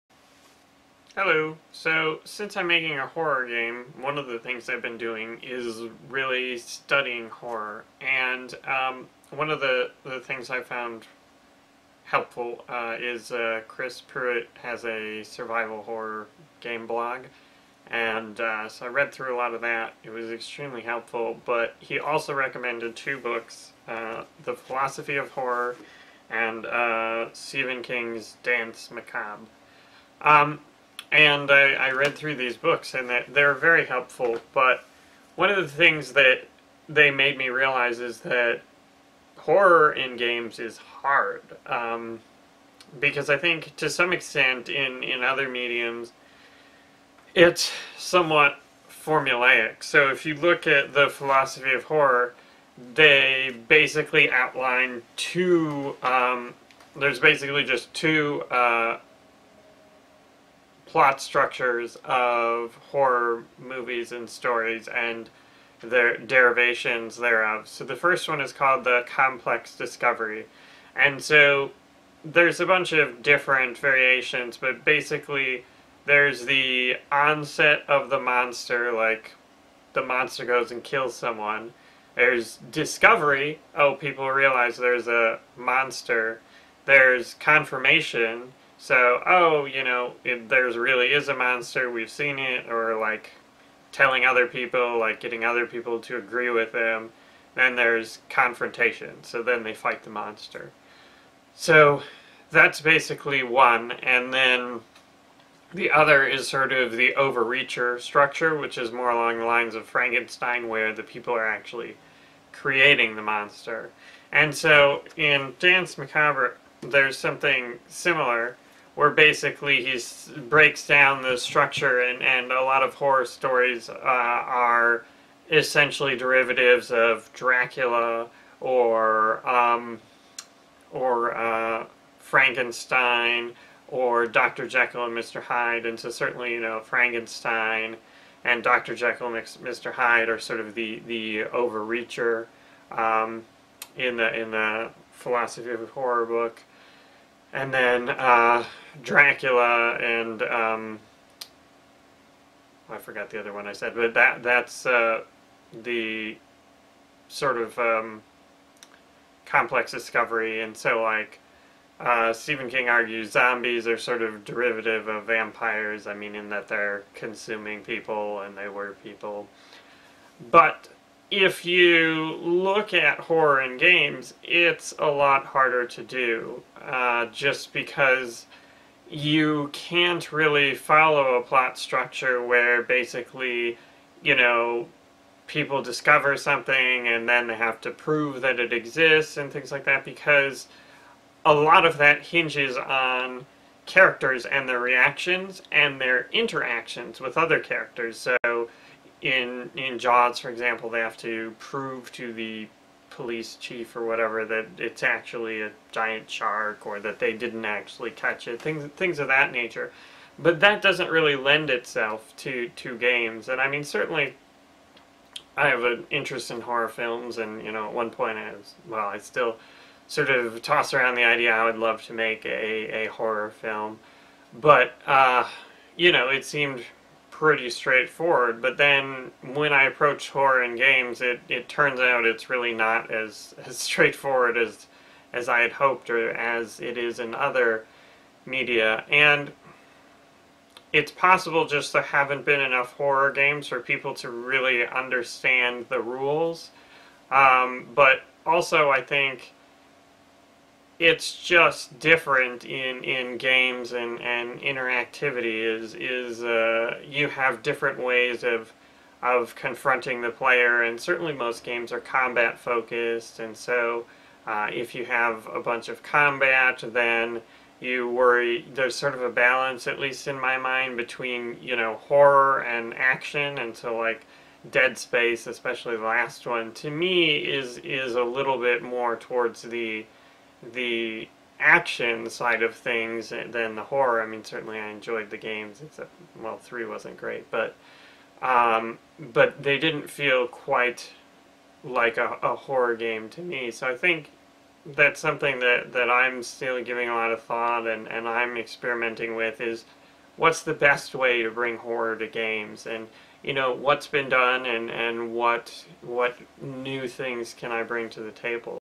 Developer Diary - 25: Horror Movies vs. Games
In this blog, I talk about the research I've done into the genre of horror and how horror in movies is different than horror in games.